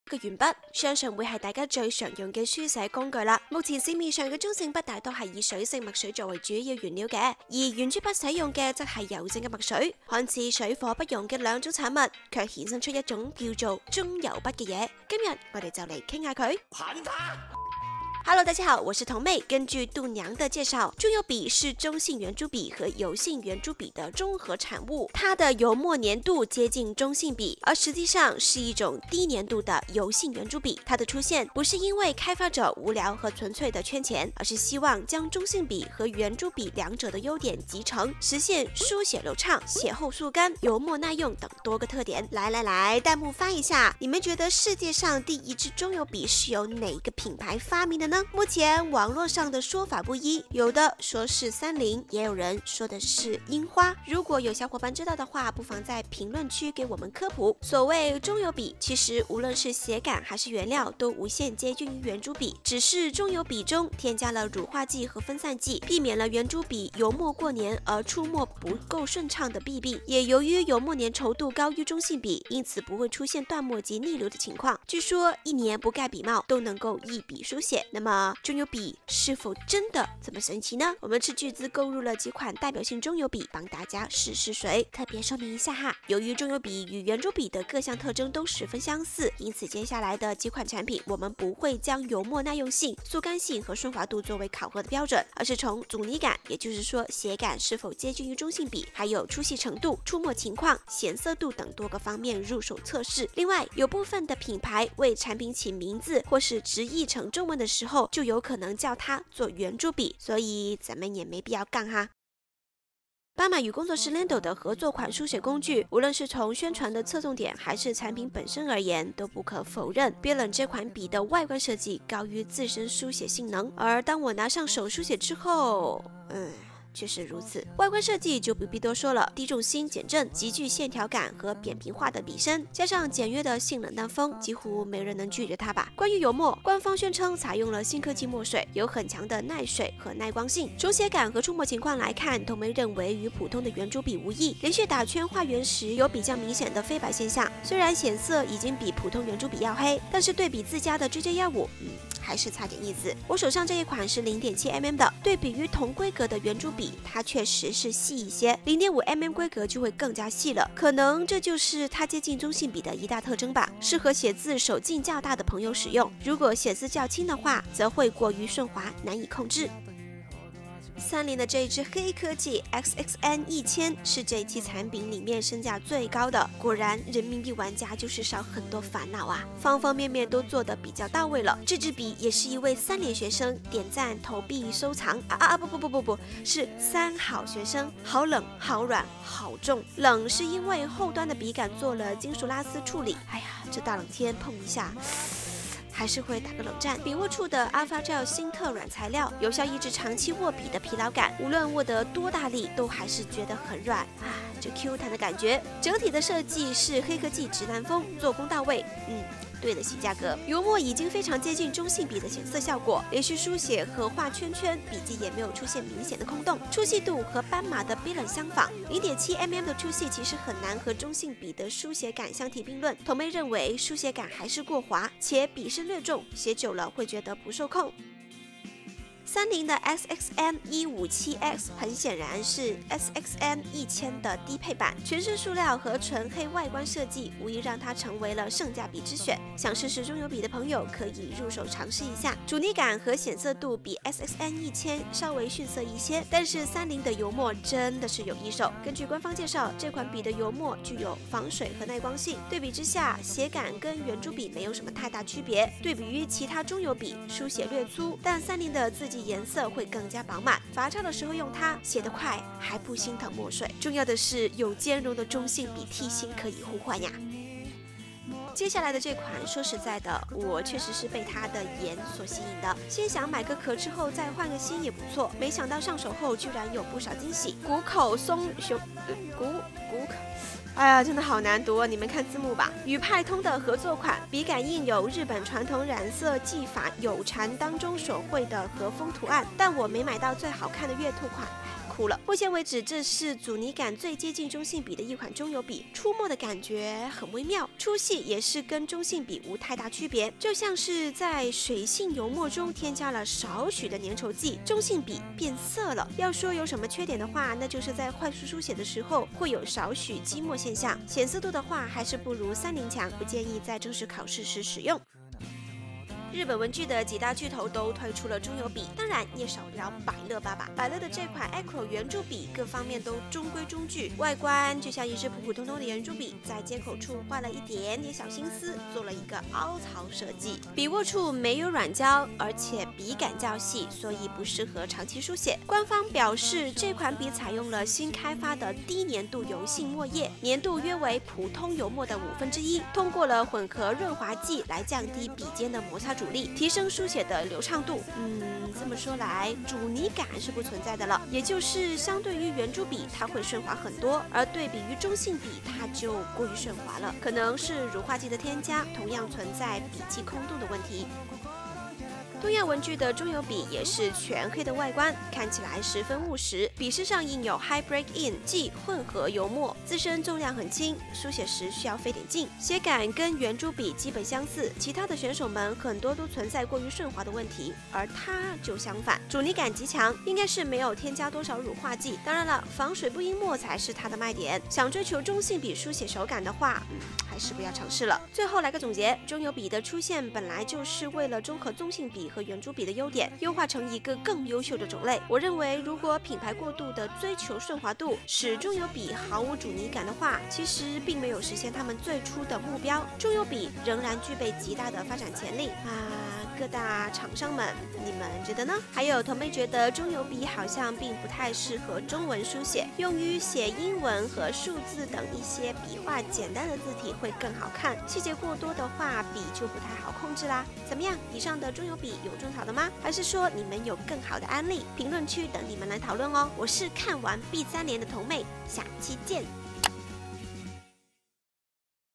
新一代书写利器？中油笔到底是什么神奇产物？【文具三分钟】第177期
试问中油哪家强
斑马极简精设计
三菱祖传黑科技
百乐东亚如何敌？